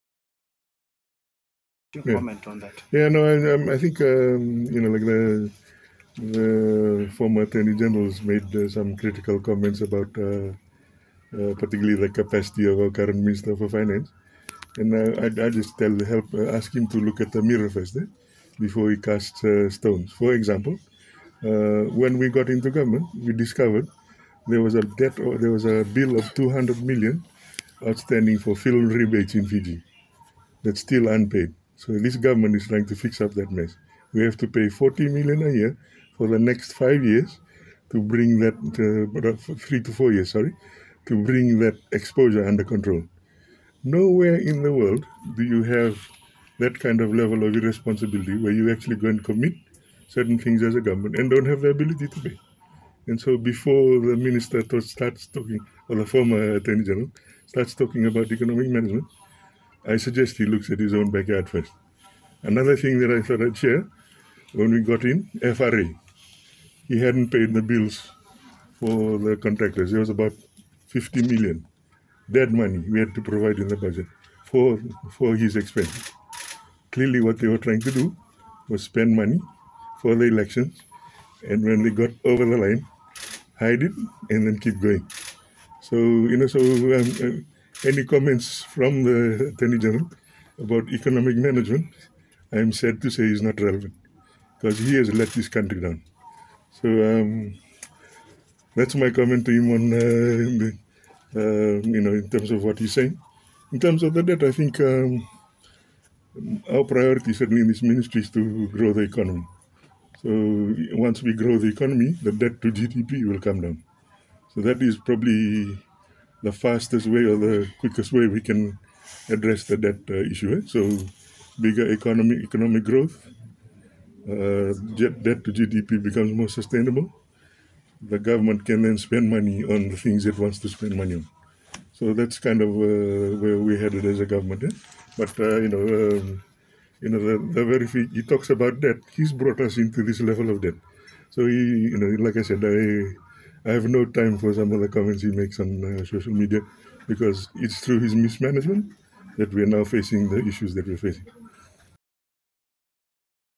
"He should look in the mirror before he starts casting stones": Kamikamica responds to Sayed-Khaiyum
The former Attorney General has made some comments about our Minister of Finance, I would just like to ask him to look in the mirror first before casting stones.

Full story here: The former Attorney General has made some comments about our Minister of Finance, I would just like to ask him to look in the mirror first before casting stones.